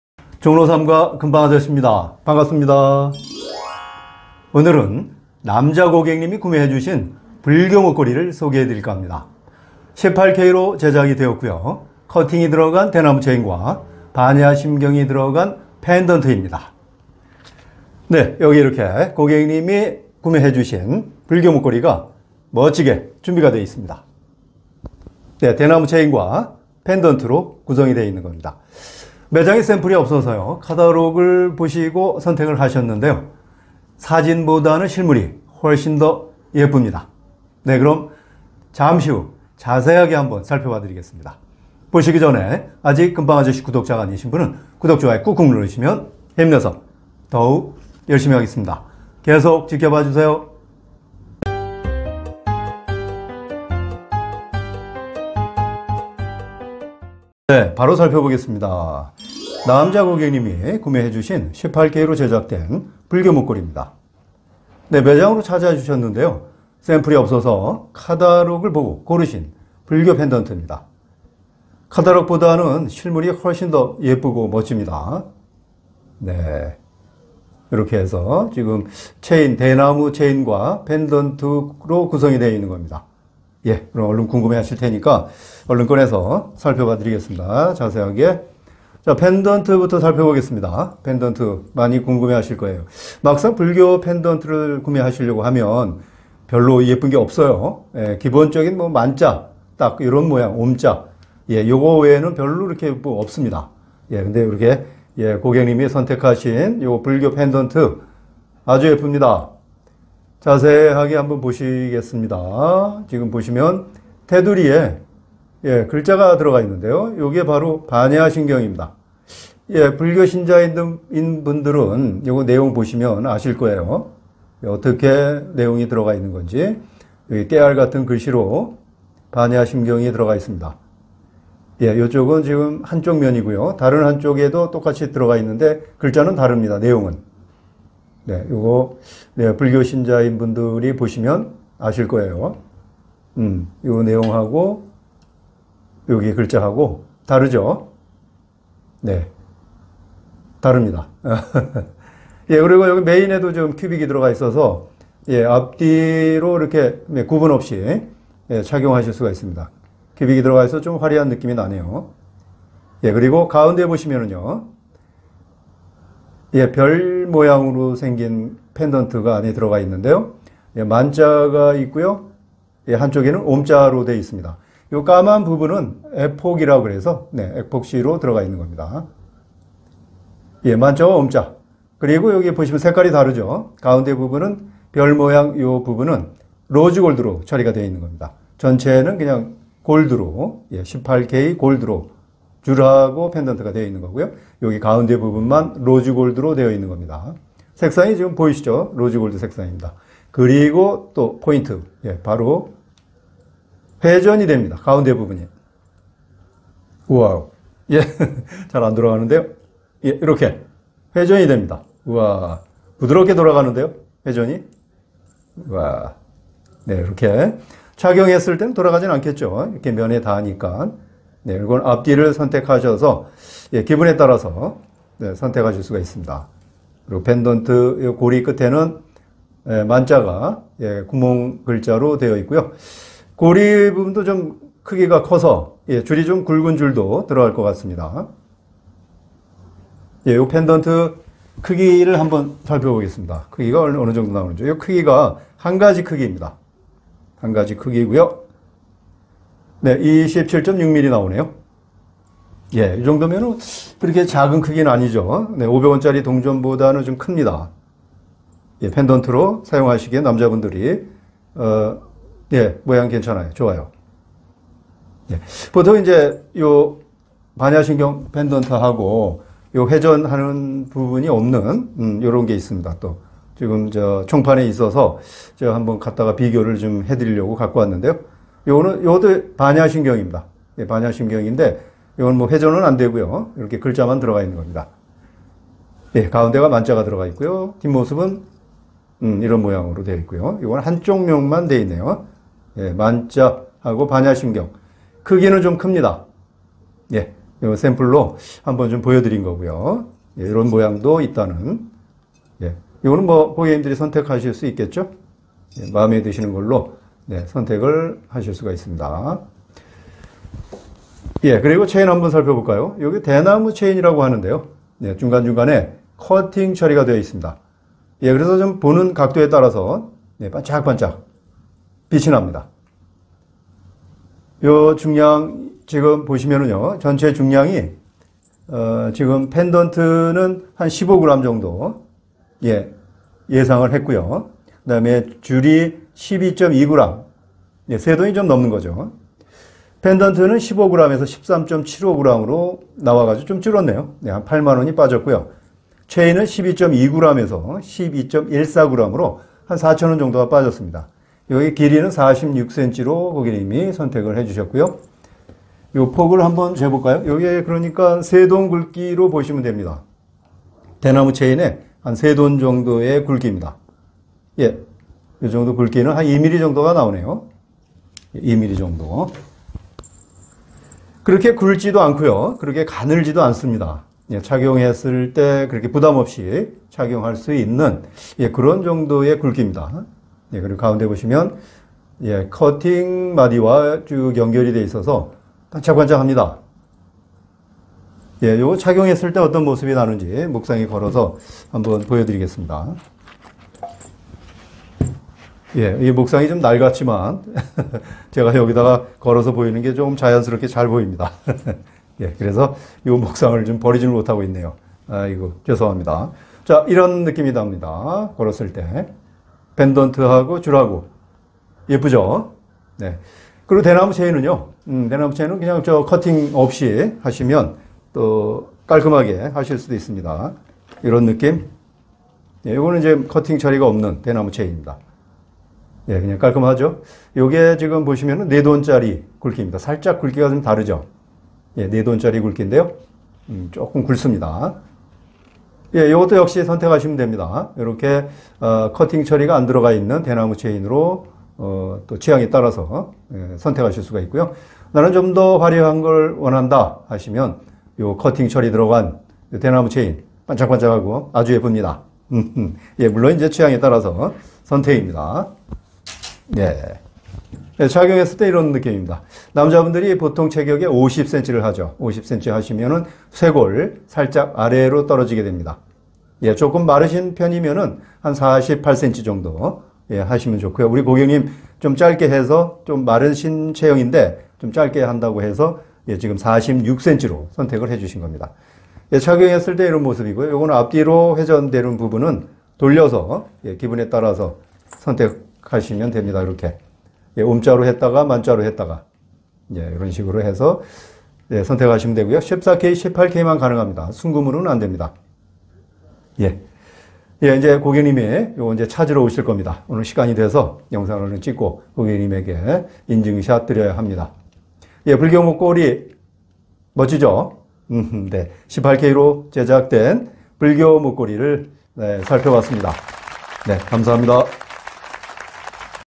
불교목걸이💖 회전형 반야심경목걸이와 커팅 대나무체인으로된 남자목걸이!🌟/주원쥬얼리 금빵아저씨
남자고객님이 구매해주신 불교목걸이~
커팅이 들어간 대나무체인과
반야심경이 들어간 팬던트~
멋지게 착용하세요~~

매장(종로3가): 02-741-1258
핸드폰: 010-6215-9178

종로3가역 10번출구 바로옆
서울귀금속상가 안  (2호) 주원쥬얼리
오전 9시 30분~ 오후 7시까지